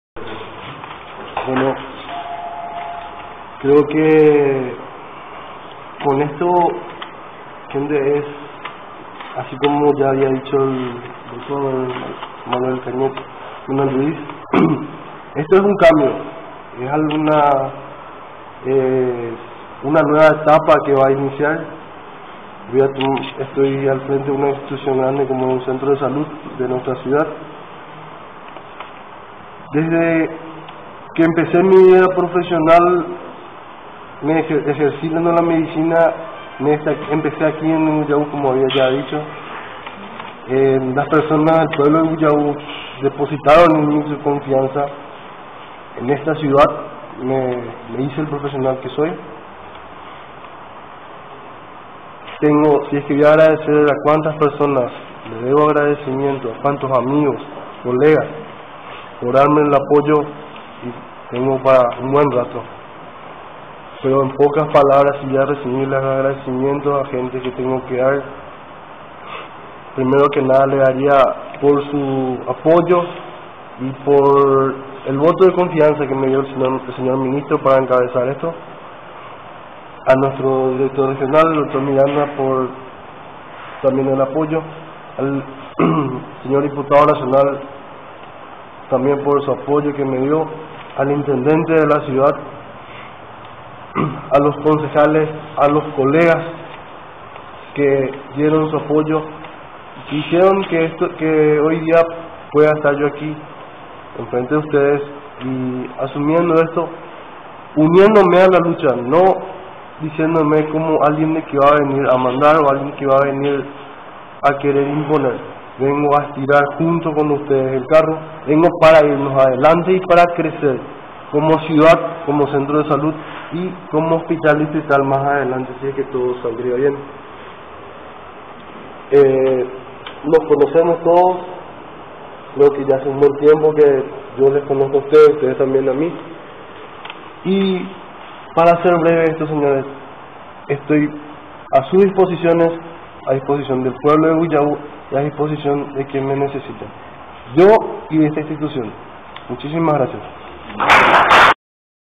Dr. CRISTIAN CENTURION , EL NUEVO DIRECTOR DEL CENTRO DE SALUD DE YBY YAU
HOY ASUME EL Dr. CRISTIAN CENTURION COMO DIRECTOR DEL CENTRO DE SALUD DE YBY YAU , HOY VIERNE  11 - DE OCTUBRE 2013